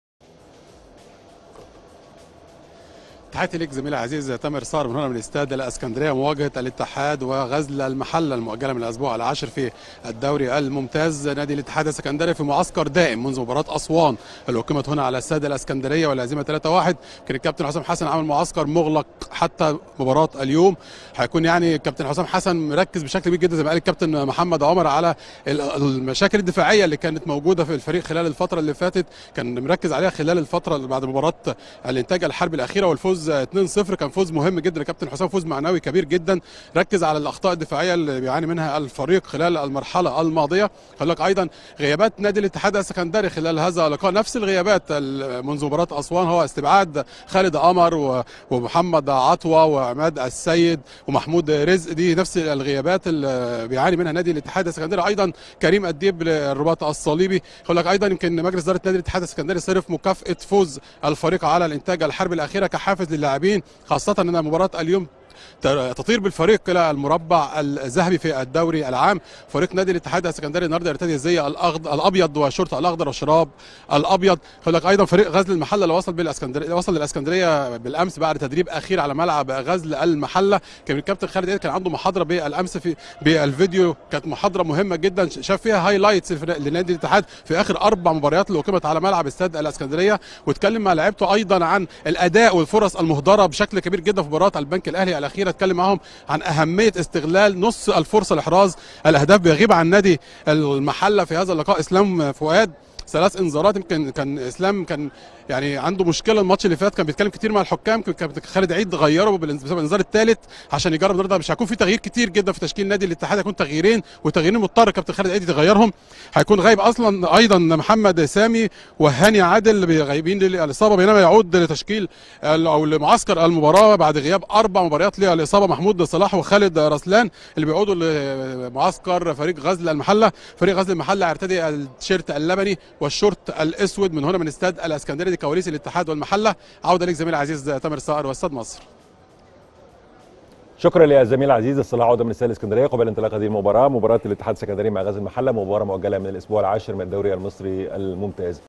ستاد مصر - أجواء وكواليس ما قبل مباراة الاتحاد السكندري وغزل المحلة من ستاد الاسكندرية
من الأستوديو التحليلى لمباراة الاتحاد السكندري وغزل المحلة

ستاد مصر